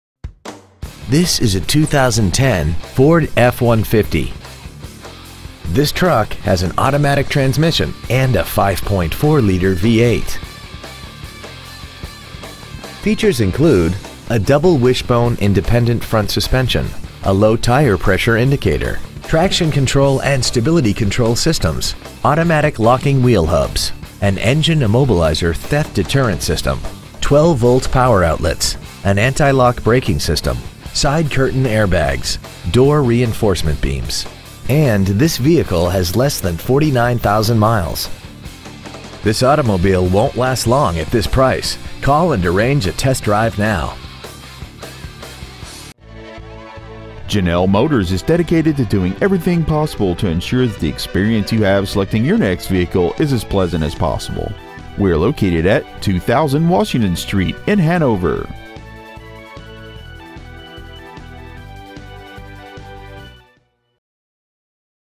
2010 Ford F-150 Hanover MA
Year : 2010
 Make : Ford
 Model : F-150
 Engine : 5.4L V8 24V MPFI SOHC FLEXIBLE FUEL
 Trans . : 6-SPEED AUTOMATIC
 Exterior : RED
 Miles : 48,293
 Interior : BLACK
 Stock : A15516